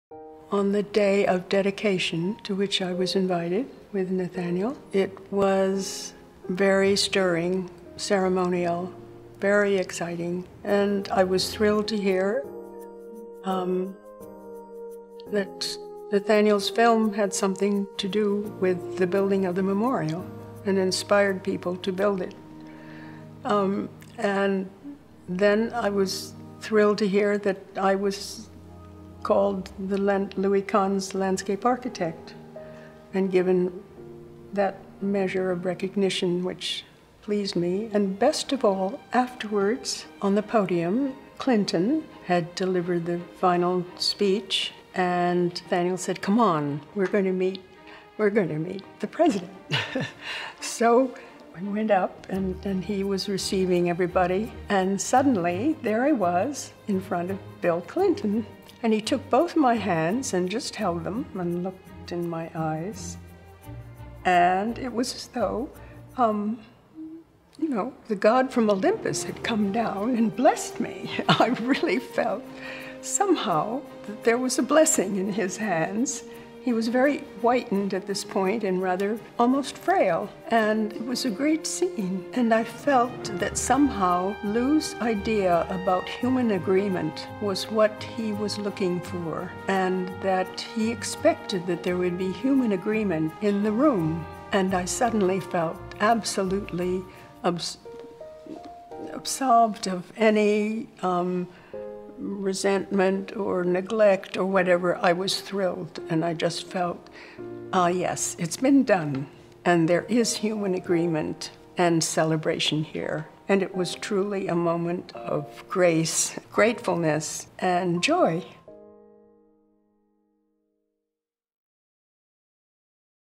Harriet Pattison on Design: Recognition, at Last [11 of 11]
The opening of Franklin D. Roosevelt Four Freedoms Park brings Pattison professional recognition, a personal sense of peace and fulfillment – and a memorable meeting with former President Bill Clinton.
Interviewed by Charles A. Birnbaum, June 2015.